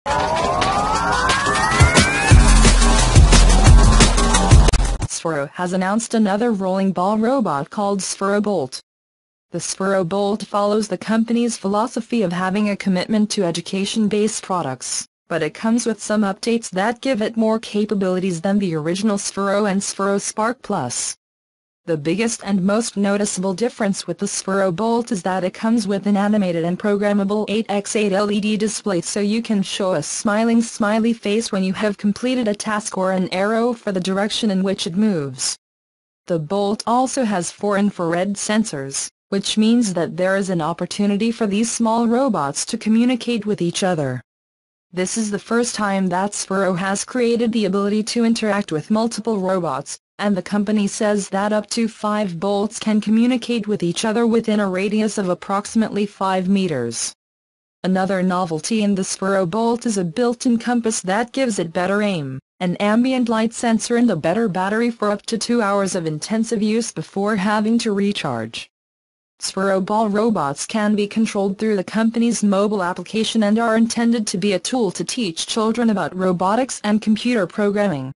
The new rolling ball robot called Sphero Bolt has a programmable LED display and infrared sensors.
Sphero has announced another rolling ball robot called Sphero Bolt. The Sphero Bolt follows the company's philosophy of having a commitment to education-based products, but it comes with some updates that give it more capabilities than the original Sphero and Sphero Spark +. The biggest and most noticeable difference with the Sphero Bolt is that it comes with an animated and programmable 8 x 8 LED display so you can show a smiling smiley face when you have completed a task or an arrow for the direction in which it moves. The Bolt also has four infrared sensors, which means that there is an opportunity for these small robots to communicate with each other. This is the first time that Sphero has created the ability to interact with multiple robots, and the company says that up to five bolts can communicate with each other within a radius of approximately five meters.

Another novelty in the Sphero Bolt is a built-in compass that gives it better aim, an ambient light sensor and a better battery for up to two hours of intensive use before having to recharge. Sphero ball robots can be controlled through the company's mobile application and are intended to be a tool to teach children about robotics and computer programming.